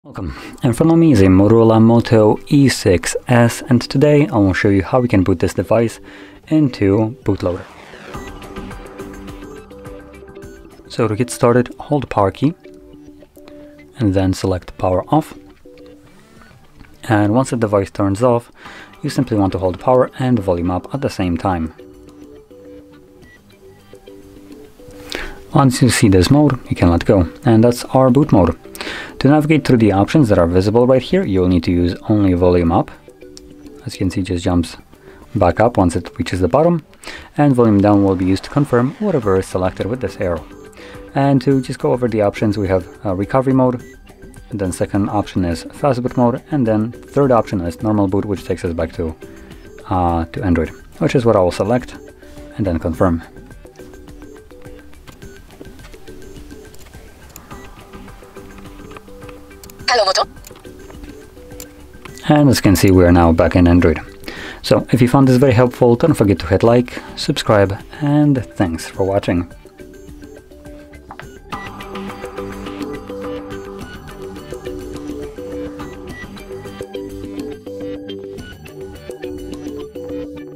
How to Open Boot Mode on MOTOROLA Moto E6S
Find out more info about MOTOROLA Moto E6s:
Here are the steps you need to follow in order to enter Boot Mode in MOTOROLA Moto E6S. Find out how to enter the secret menu and start Boot Mode by using the right button combinations.

How to open boot mode on MOTOROLA Moto E6S? How to enter boot mode on MOTOROLA Moto E6S?